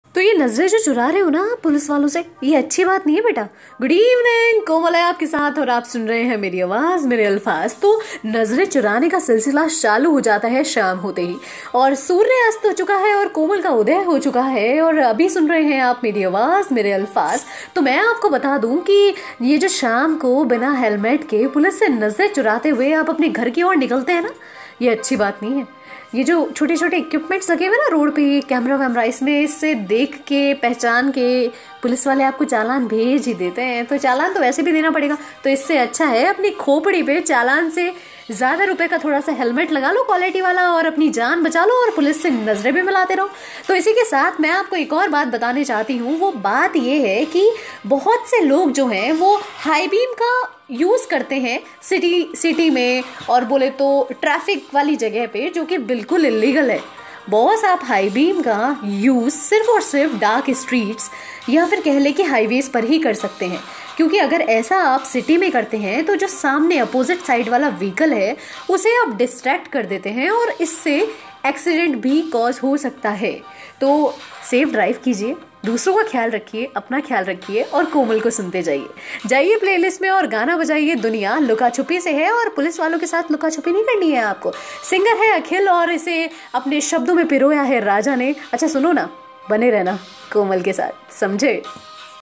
Khoopdi par halmet lagao aur ride ko safe banao| high beam | saferide | komal
Chaalaan toh vaise bhi katna hai. toh halmet lagao aur jindagi ko safe bnao.